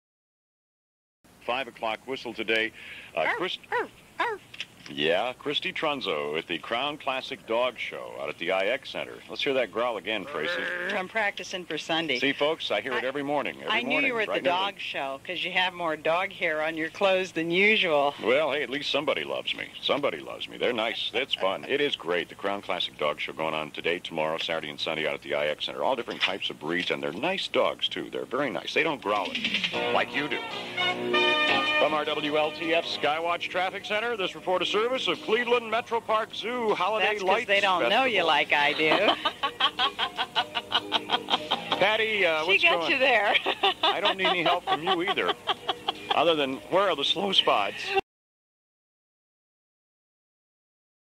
Bruce Ryan and Tracy St. John promote the Crown Classic Dog Show on WLTF
Bruce Ryan and Tracy St. John promote the 5 o'clock Whistle and the Crown Classic Dog Show at the IX Center.